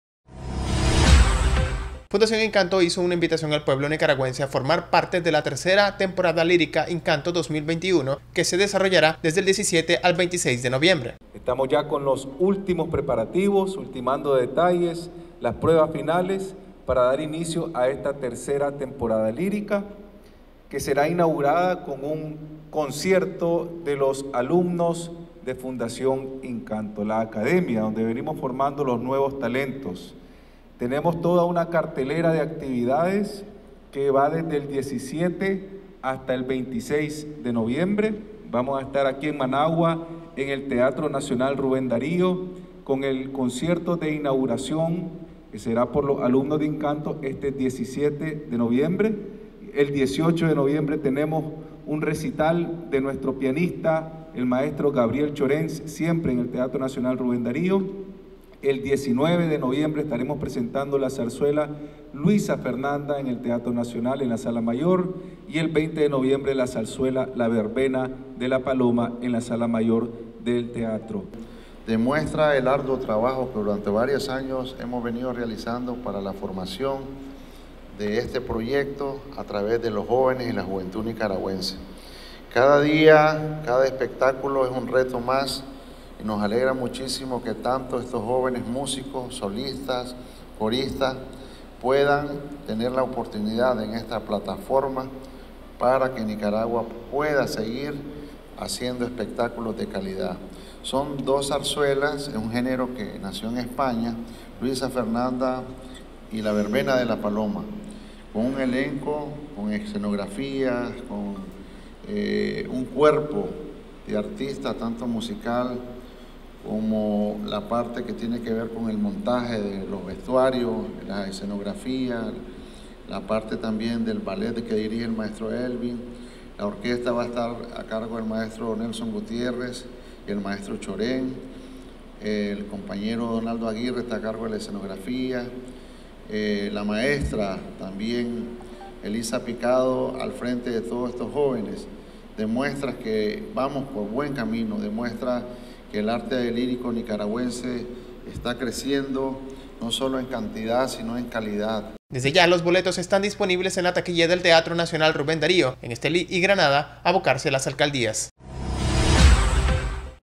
Invitan a disfrutar de la Tercera Temporada Lírica Incanto 2021
Fundación Incanto hizo una invitación al pueblo nicaragüense a formar parte de la Tercera Temporada Lírica Incanto 2021 que se desarrollará desde el 17 hasta el 26 de noviembre.

El compañero Laureano Ortega, Director de Fundación Incanto, expresó que están con los últimos preparativos para dar inicio a esta Tercera Temporada Lírica, la cual será inaugurada con un concierto de los alumnos de Fundación Incanto.